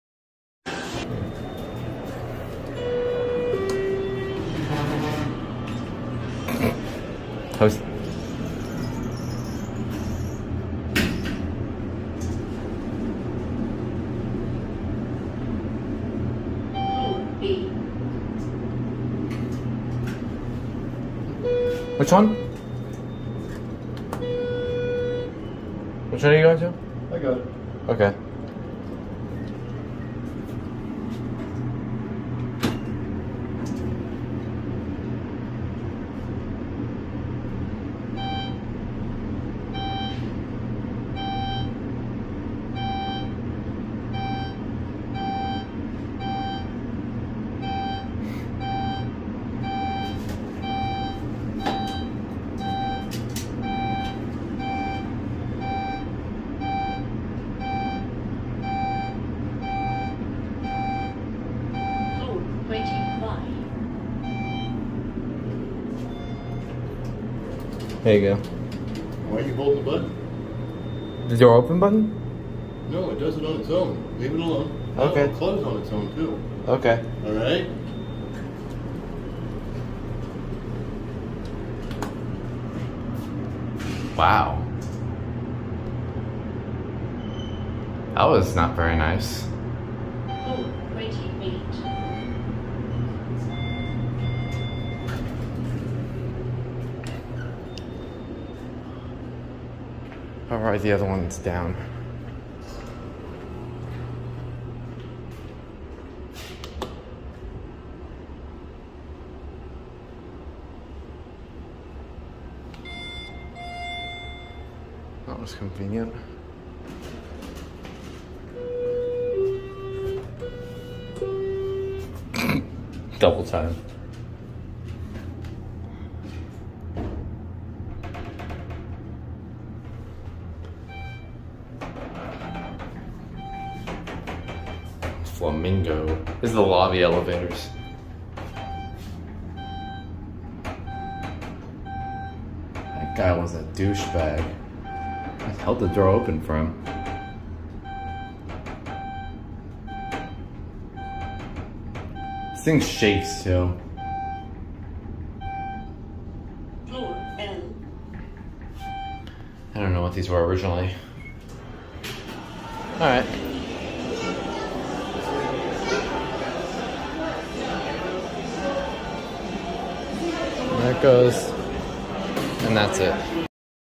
KEVIN ALERT: Schindler 6400NA Traction Lobby Elevators @ Flamingo, Las Vegas, NV
I have never in my 5 years of elevator photography encountered somebody this rude. He was clearly in a mood. Just by filming alone he was giving me dirty looks. Just as I should for others and my parents request me to hold the door for others and that includes elevators. Other elevator enthusiasts do this too. But this guy was not having it at all, bonus points for him appearing to be a manager or security guard. I was just holding the door open for him and he gave me an attitude for it and said it does it on its own. I also asked him what floor he was going to but he immediately declined with a dirty look at me while I was trying to do something nice. Overall he was a naughty person and gives a bad impression on the facility.

The elevators themselves were not that interesting. They were just Schindler 6400NA's that obviously modded something else.

Tech Specs:
Brand: Schindler
Originally: Otis
Maintenance: ??
Model/Fixture: 6400NA (MAD BS Moon)
Type: Traction
Floors Served: B, L, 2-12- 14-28
DT Rate: 2/10 Not good